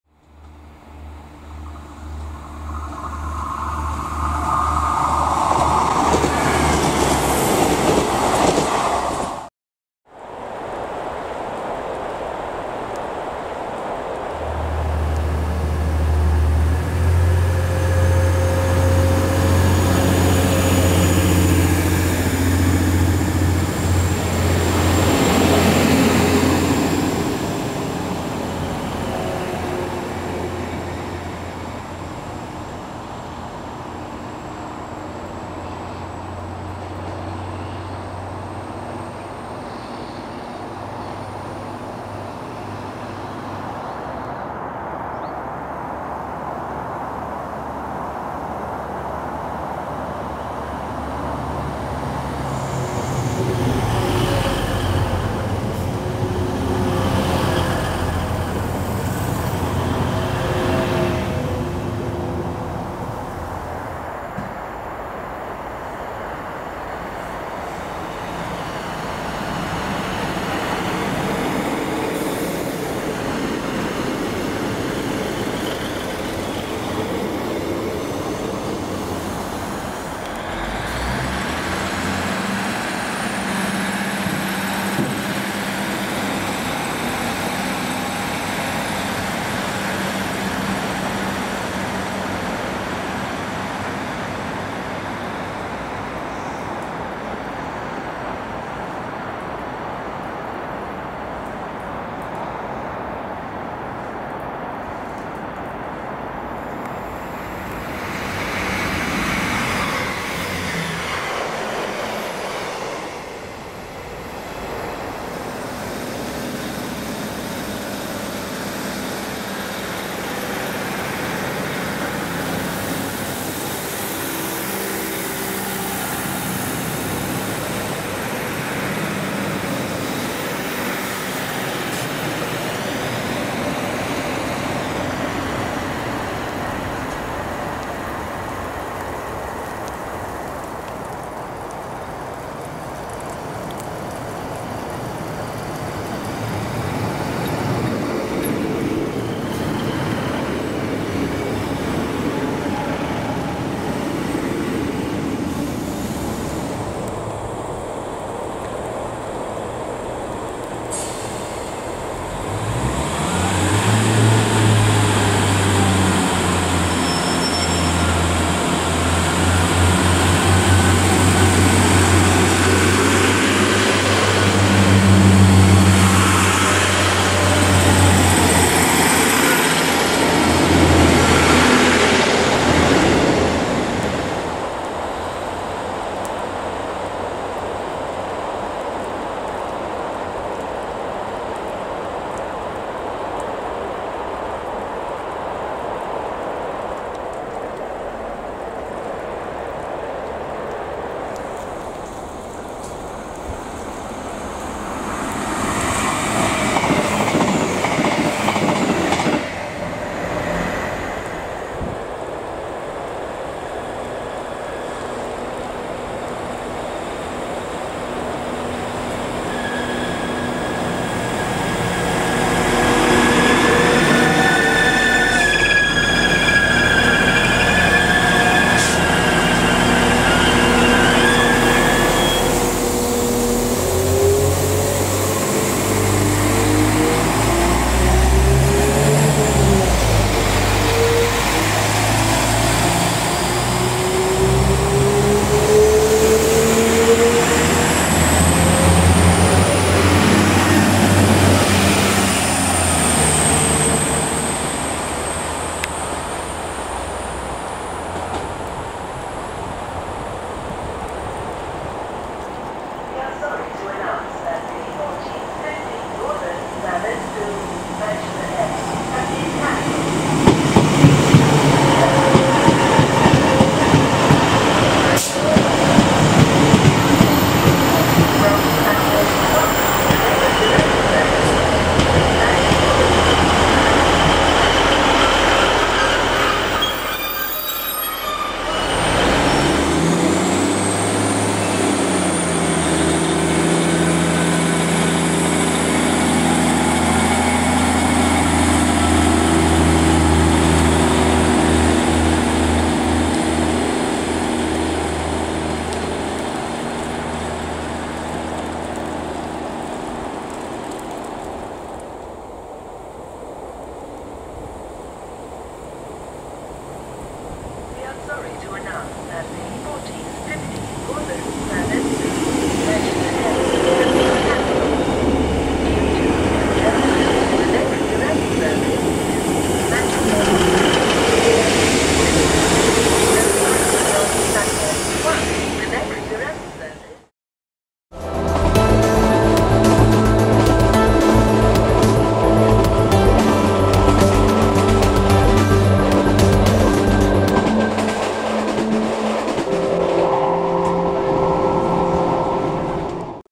Birchwood Station 25/9/23
Birchwood Station is on one of what was four routes between Liverpool and Manchester, this one in question is the line that runs between these two vibrant cities via Widnes.

The station is managed by Northern Rail and has two Platforms

Northern Rail operate most National Rail services here with 2 trains a hour either heading towards Liverpool Lime Street or towards Manchester Oxford Road; services here are provided with either Class 150/156 and Class 195 Diesel Multiple Units.

East Midlands Railway call here with services heading towards Norwich via: Manchester, Sheffield and Nottingham or towards Liverpool Lime Street, these services are operated using Class 156 and Class 158 Diesel Multiple Units; these services run in either direction on a mostly hourly frequency during the day with only peak time EMR services calling here.

Transpennine up to December 2023 also operated hourly trains that call here these formed part of the Liverpool Lime St- Cleethorpes services, using Class 185 Diesel Multiple Units,